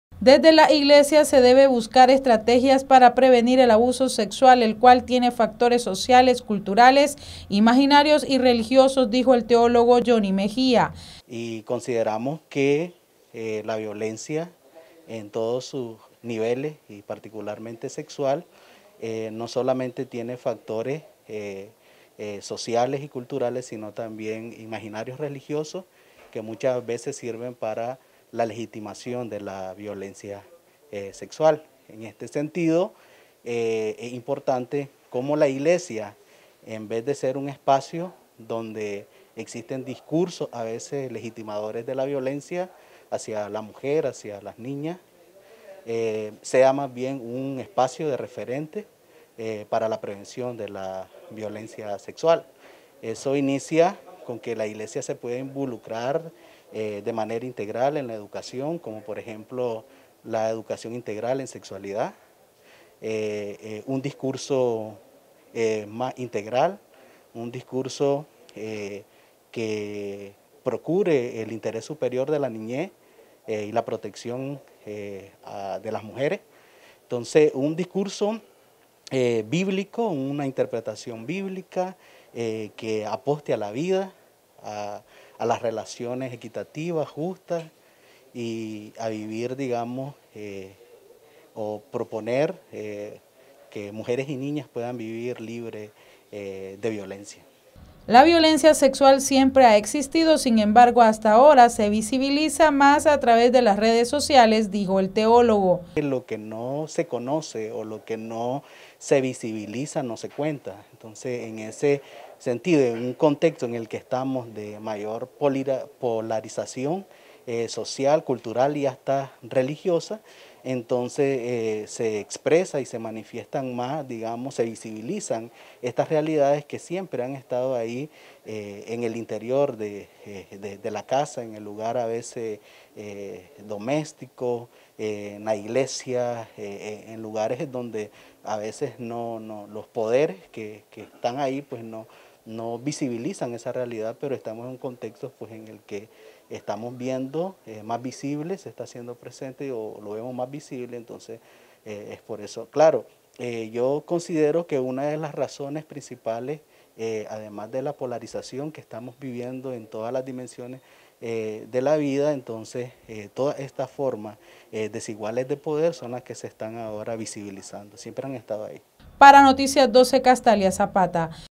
Iglesias deben sumarse a la prevención de la violencia sexual
NosResistimosAlEmbargo Iglesias deben sumarse a la prevención de la violencia sexual, dijo el teólogo Yonny Mejía